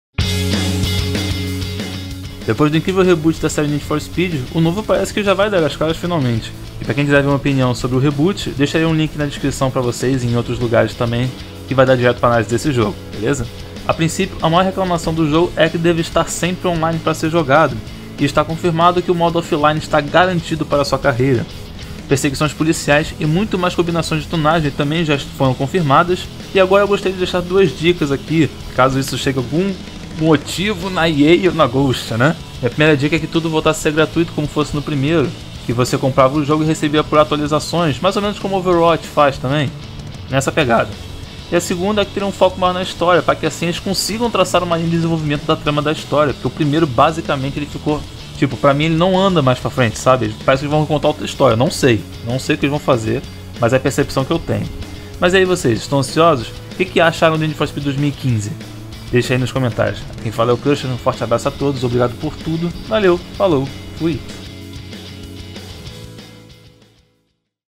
🎧 NFS 2017 - Retribuição: Novidades Saíram Finalmente
Não esqueça de avaliar, comentar, se inscrever para mais conteúdos e de nos seguir nas \/

Resumo
Hoje na pauta da equipe do TorqueTotal, Cruxer traz o quanto antes todas as informações até o momento do novo jogo da franquia need for speed. Não é muito mas já pra perceber que os fãs foram ouvidos.